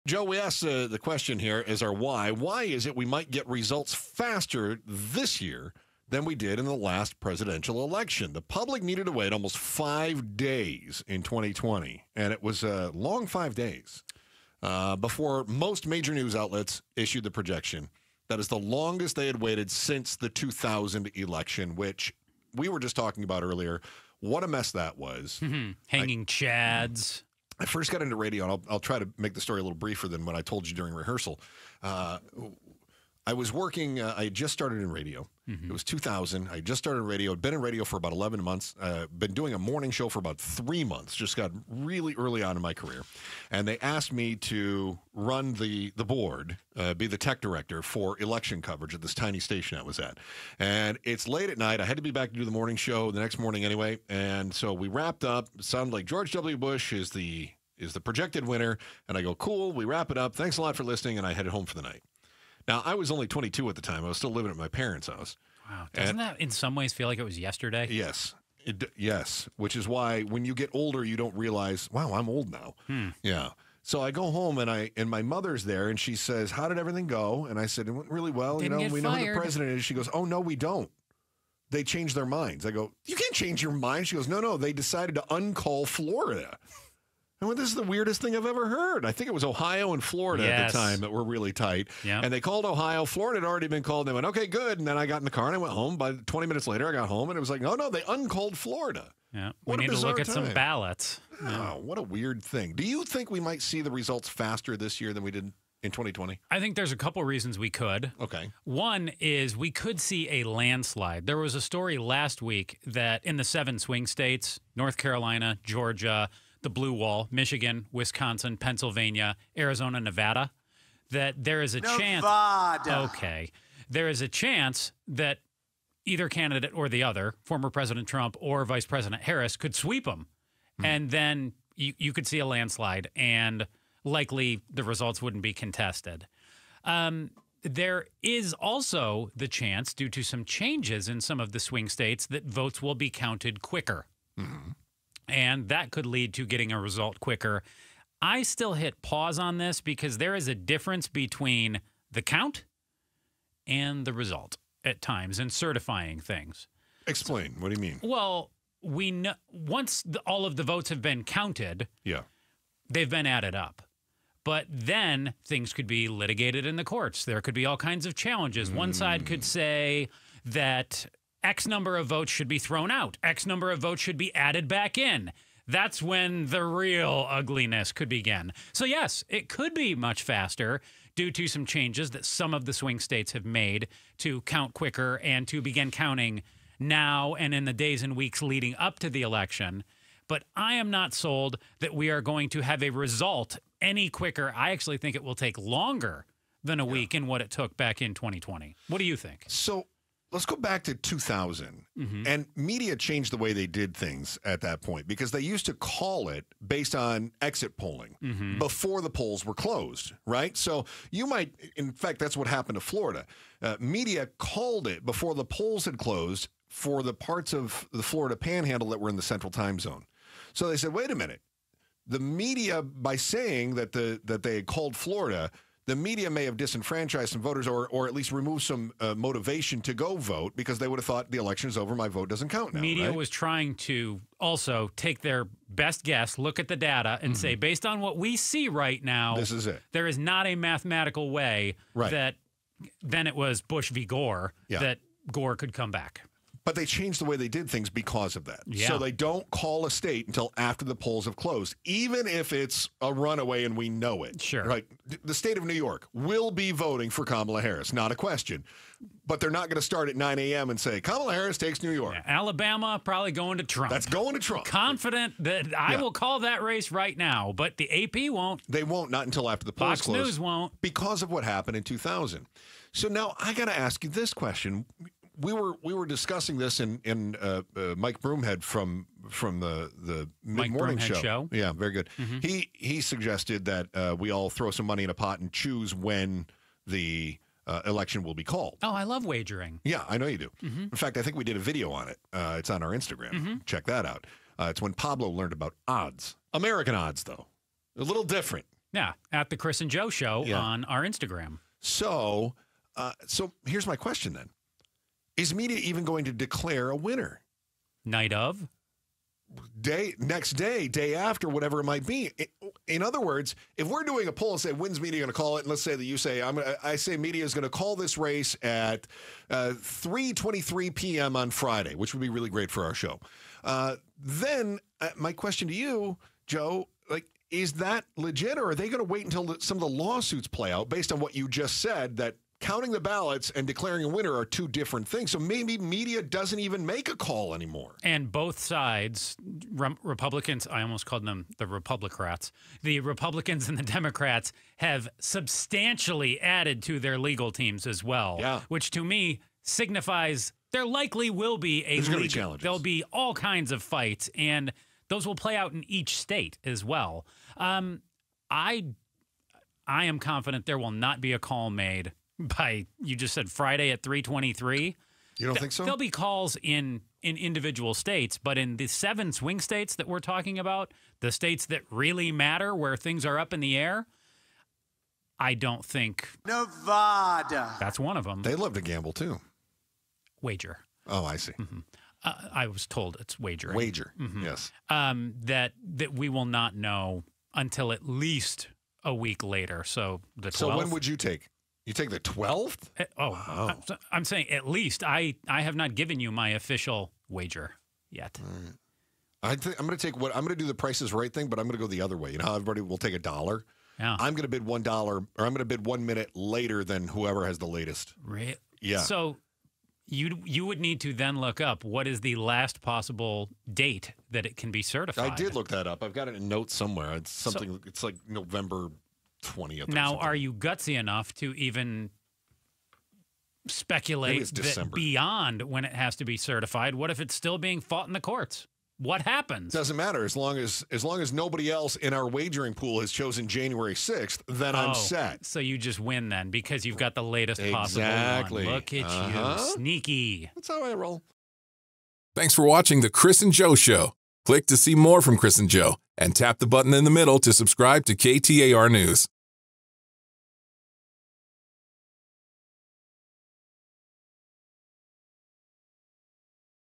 Why we might get results faster than the last presidential election
Why we might get results faster than the last presidential election. Chris & Joe break it down.